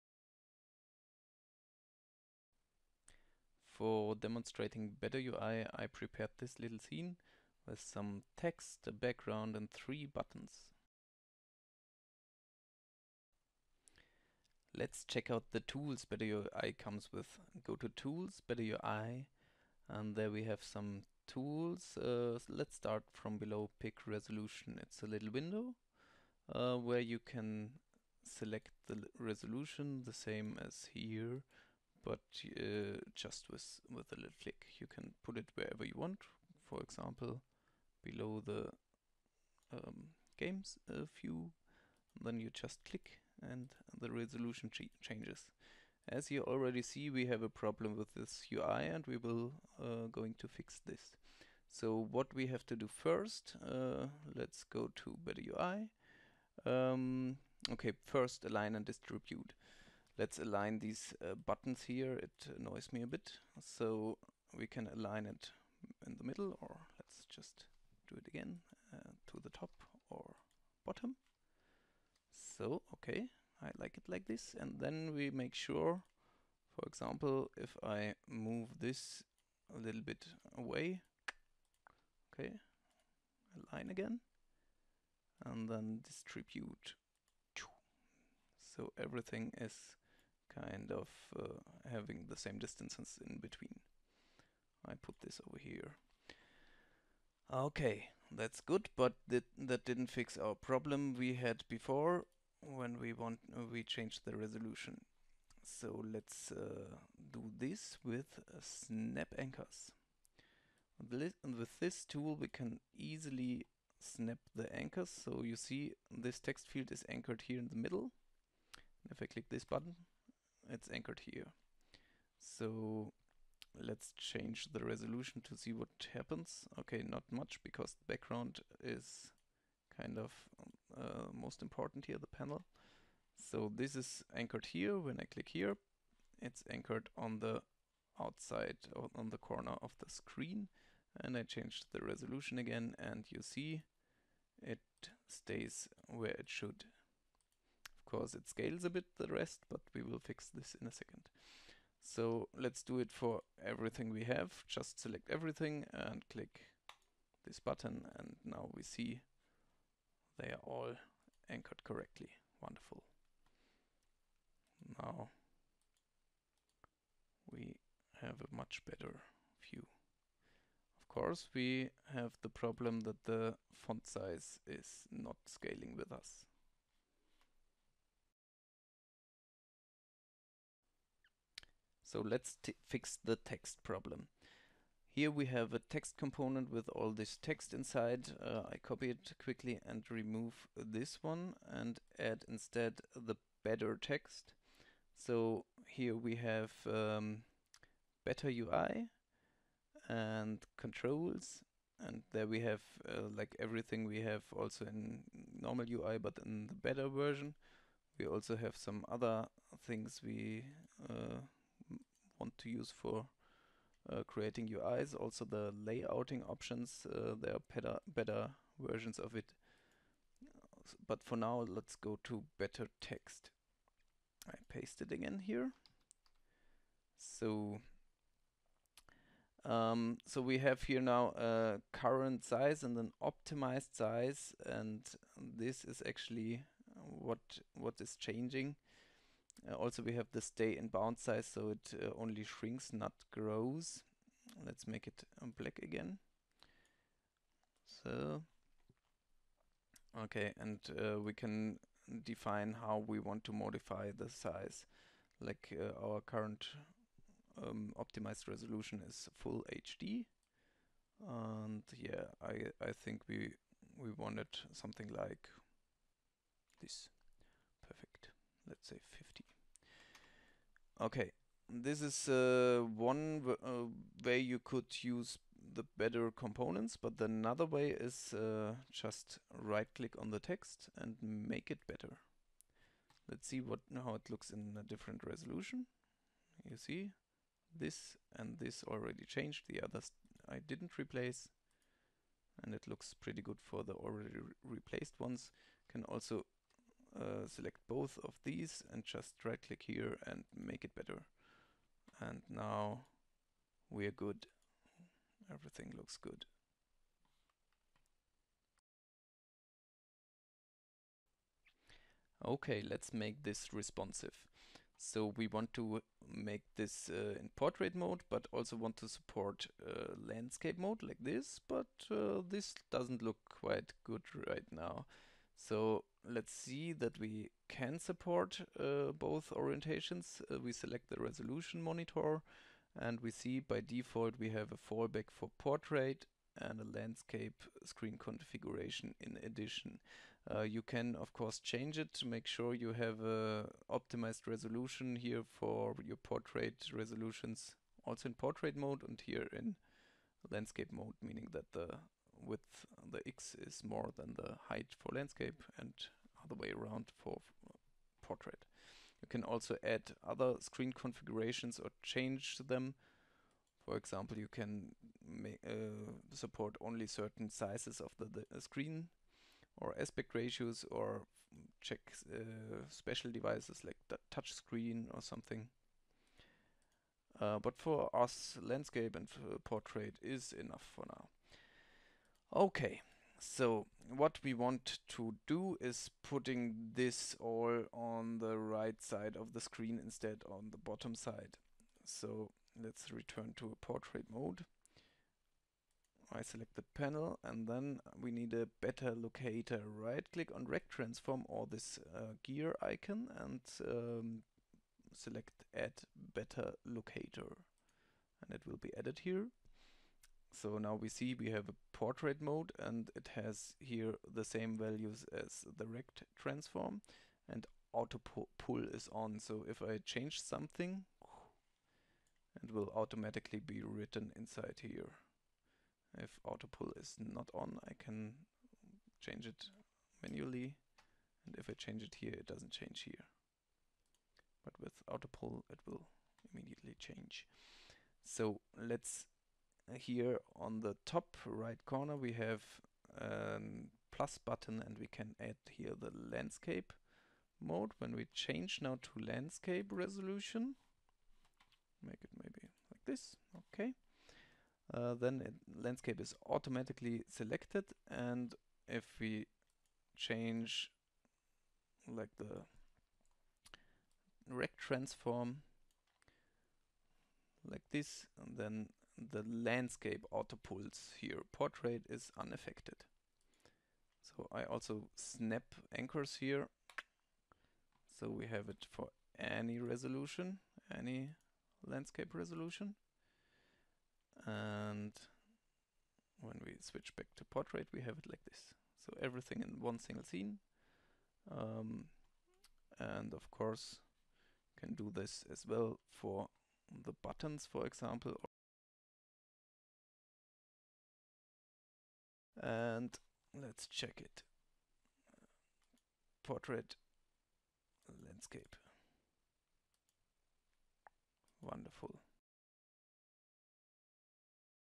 Better UI - resolution overview
About the resoultion / responsive design part of the Unity Asset "Better UI".